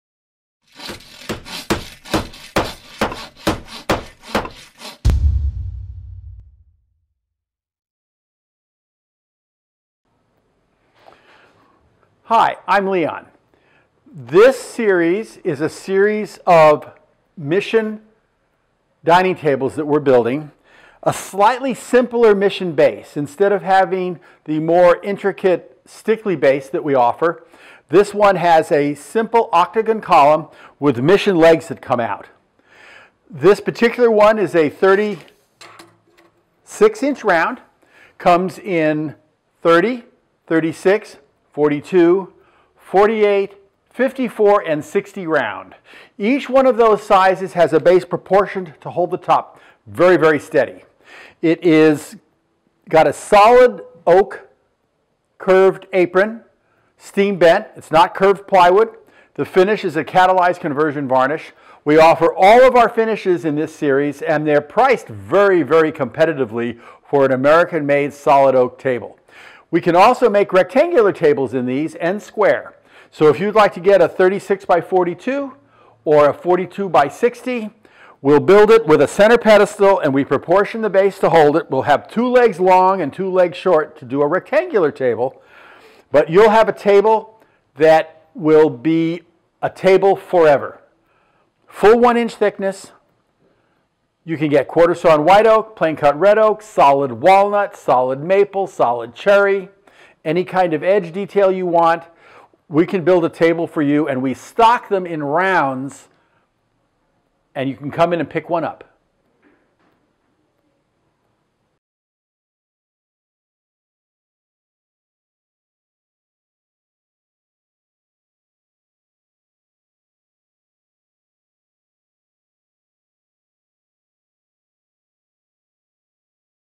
Barn Furniture Mission Round Dining Tables 30" 36" 42" 48" 54" 60" 72" with or without leaves
6206 Sepulveda Blvd
Van Nuys California 91411

Available in round, rectangle, oval or square! Name the size shape wood or color. Proudly made in the USA so we can give you choices quality and value to last a lifetime.

Oak Maple Cherry Quarter Sawn White Oak in any color.
This series of Amish, hand crafted round tables are in stock in Red Oak and Quarter Sawn White oak in our Michels finish shown in video. The second line will take you to more round tables. The third link will take you to much of our dining furniture.